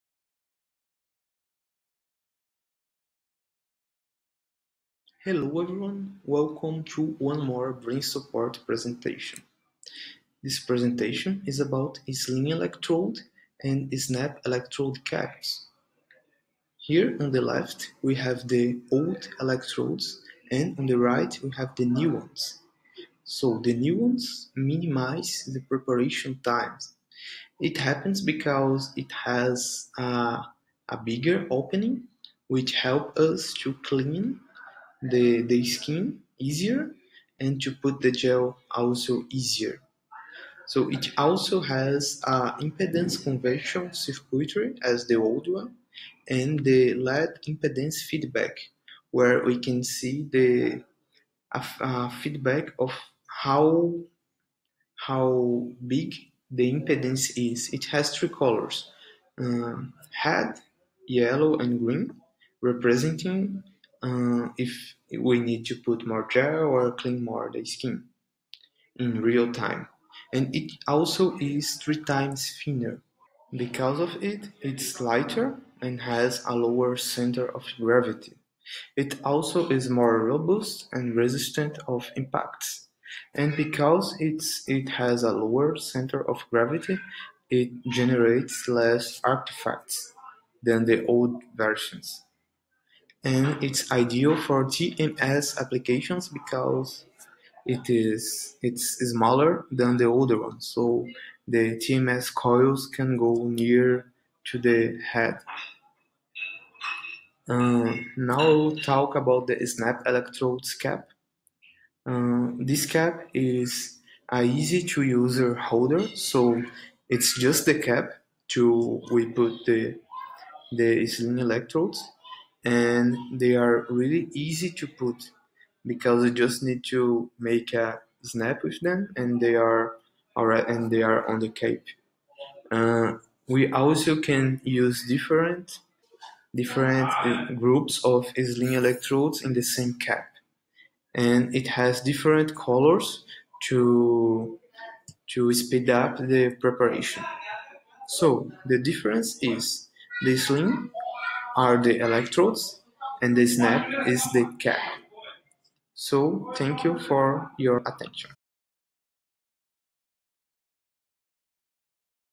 Slim electrodes and Snap electrodes cap
Presentation about slim electrodes and snap electrodes cap from. An active EEG solution developed by Brain Products.